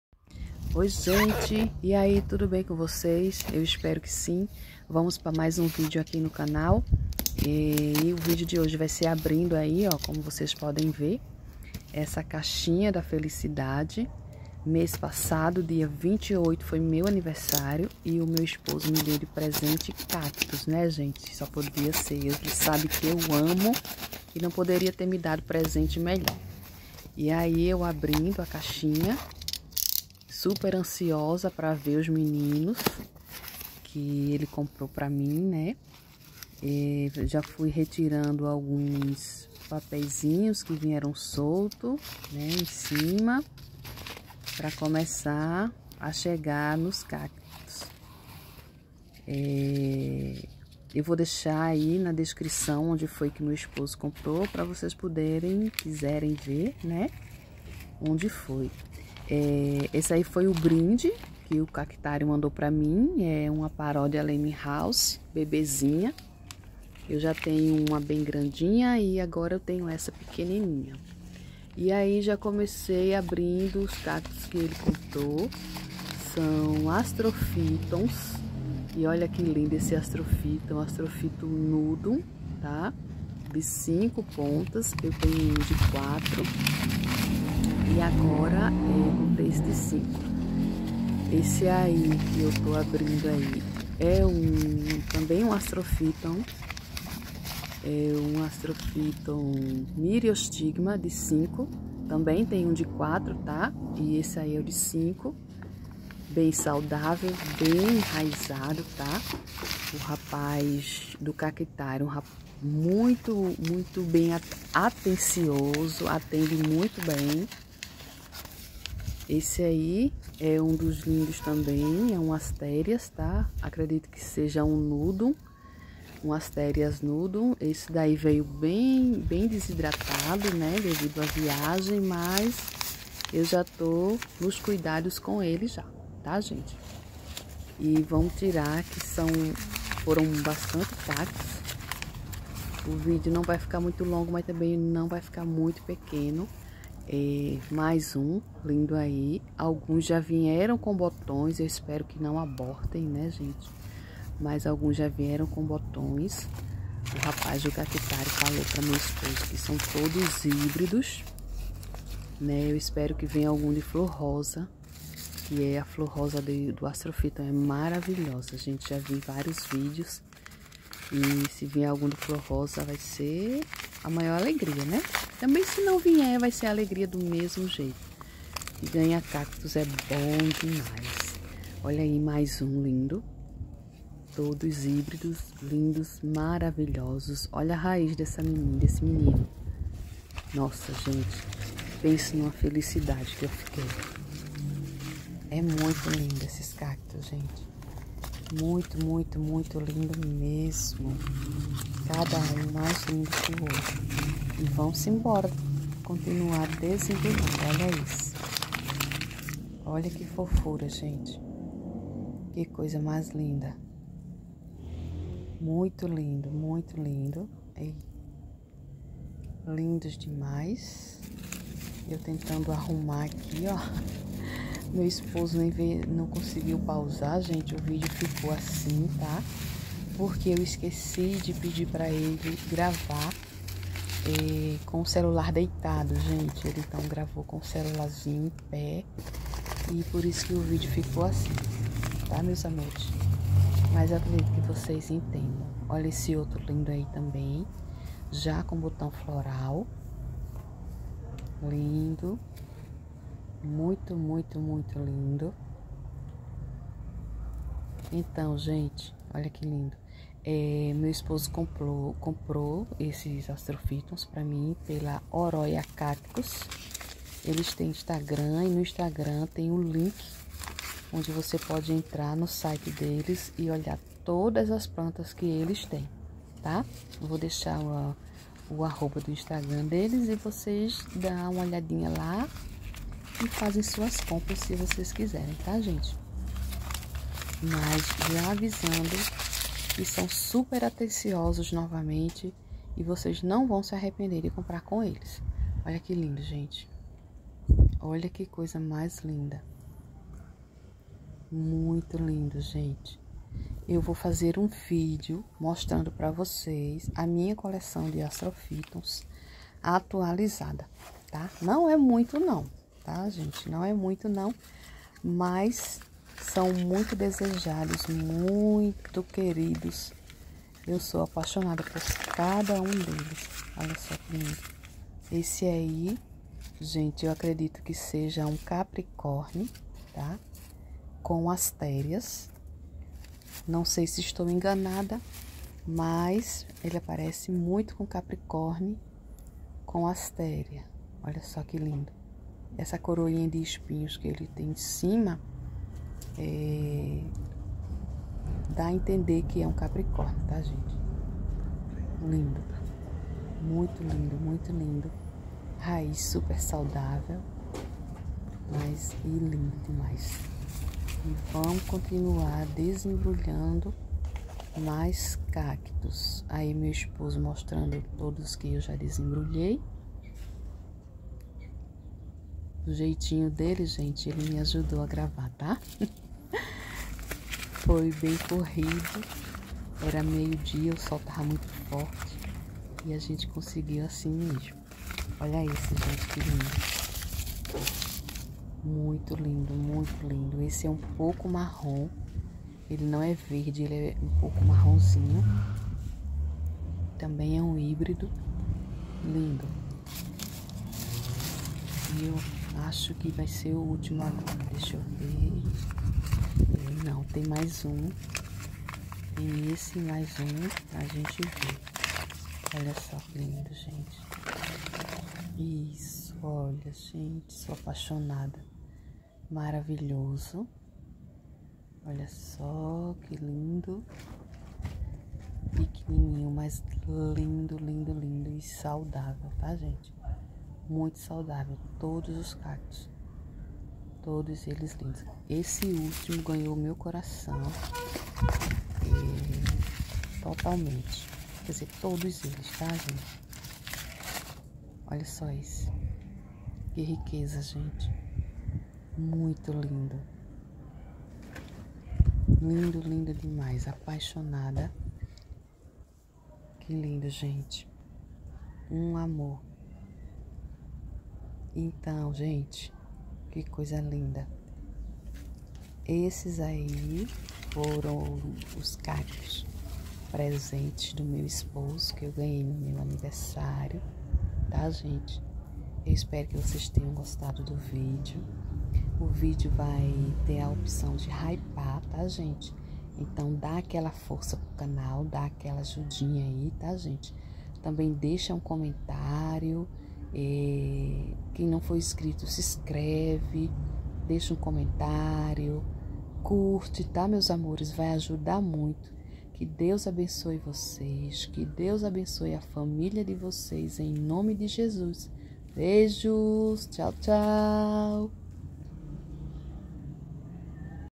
caixa da felicidade astrophytum 💚🥰🌵🌵
Astrophytum lindos chegados do cactario
oroyacactos💚🌵